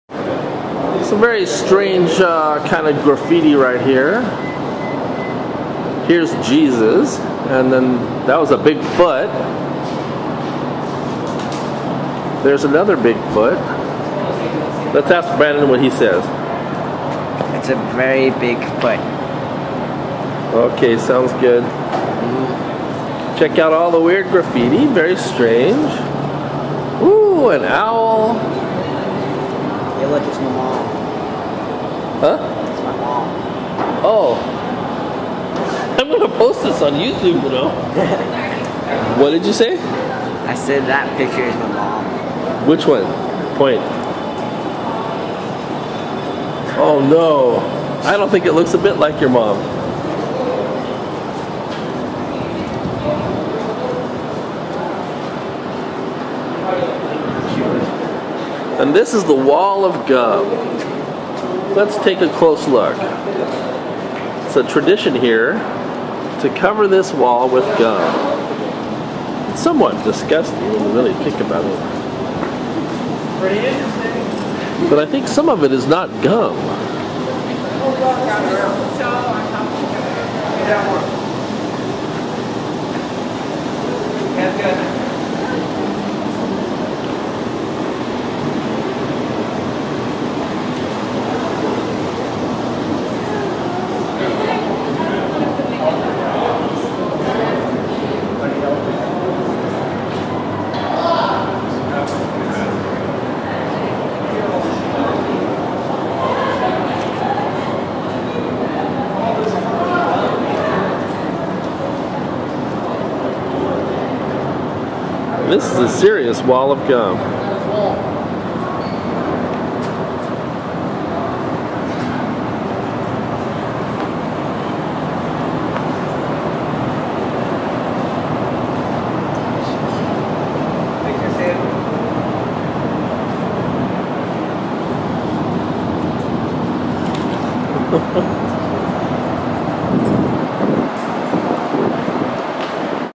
wall covered with gum, pike place market
In post alley, below the pike place market, there is a theater. tradition has it to stick gum on the wall there!